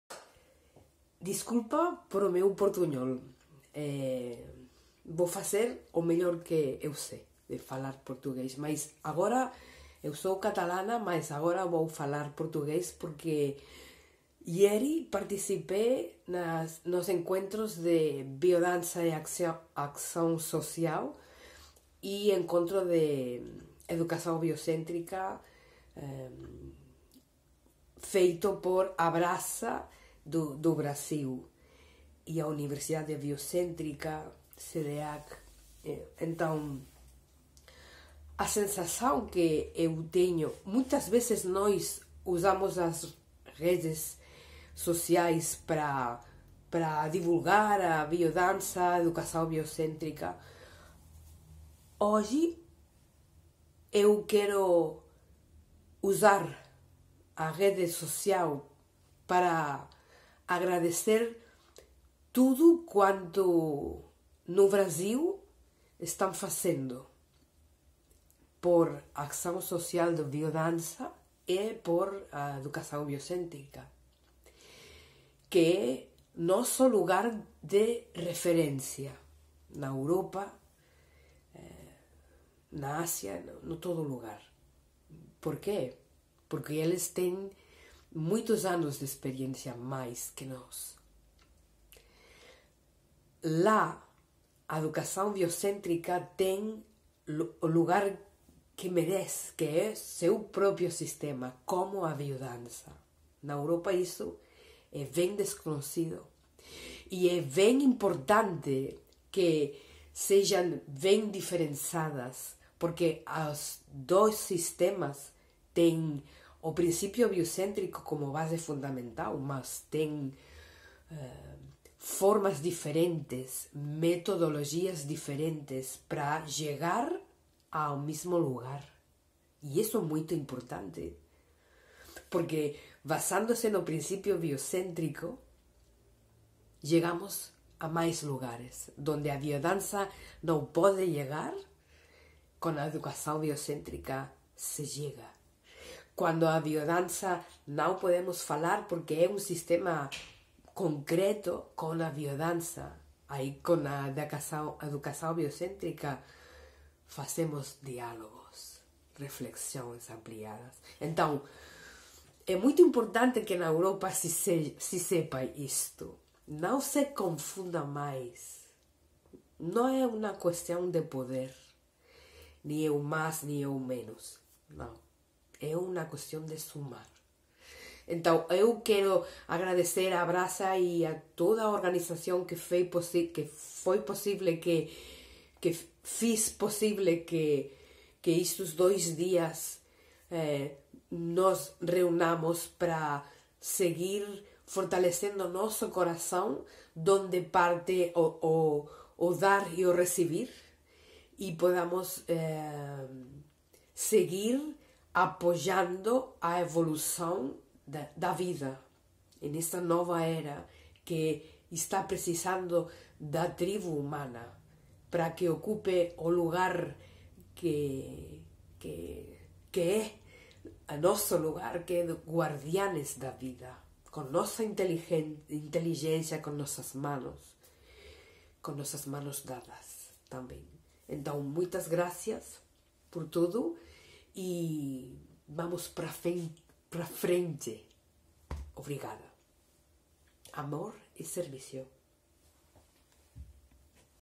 A matriz biocentrica do Brasil. Um referente do bemfazer e bemamar
Este video surje de mi corazón agradecido por tanto recibido en estos dos dias de encuentro (25 y 25 de noviembre 2022- IX Encontro de Biodança, Açao Social e VI Educaçao Biocêntrica)

Desde el minuto zero todo fue rodado, puntualidad, orden, silencio, atencion, entrega. las calidad de todos los trabajos presentados fue totalmente inspiradora. El trato recibido como ponente, y el rigor humano brillaron presentes en todos los rostros y en todos los actos k se fueron desarrollando.

Pudimos presenciar ela Obra sintesis de dos candidatos a la titulación como Facilitadores de Biodanza, a la devolución de sus tutoras con consejos maravillosos que apoyaban el buen hacer del equipo. Nunca me puedo cansar de agradecer tanto que aprendi con todos ustedes.

Mi inmensa gratitud. Sigo adelante foertalecida en mi empeño de servir a la Vida de la mejor manera que se.

Gratidāo🙏🏽